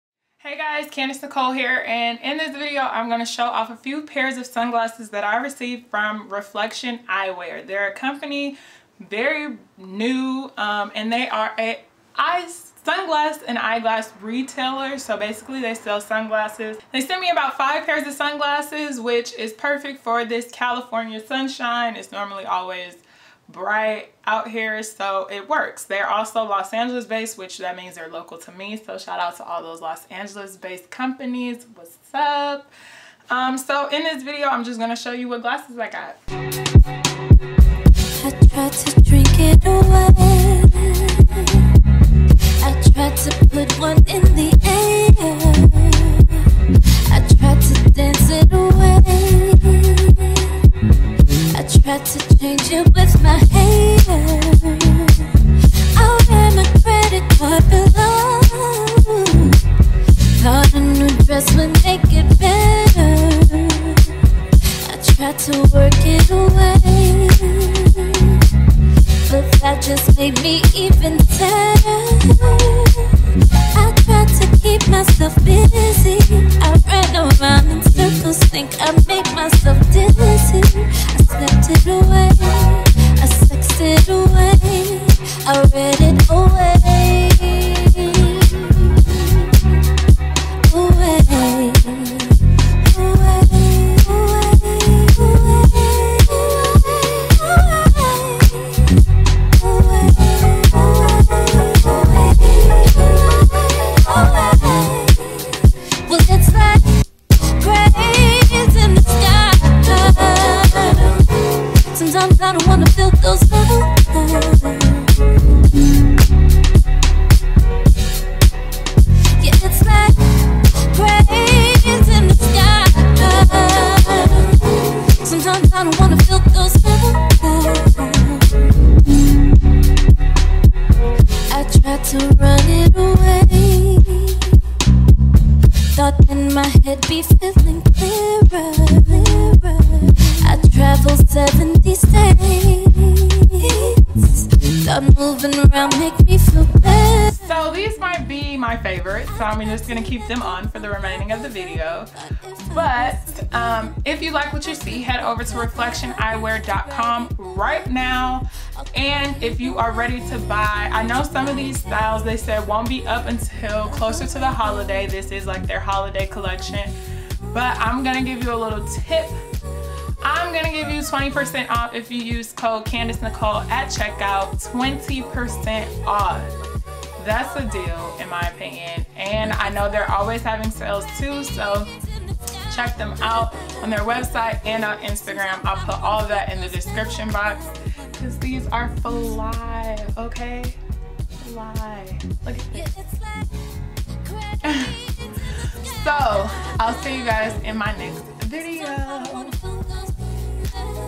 New Sunglasses From Reflection Eyewear!
Reflection Eyewear (IG: @reflectioneyewear - reflectioneyewear.com) sent me 5 pair of shades to show off their holiday collection!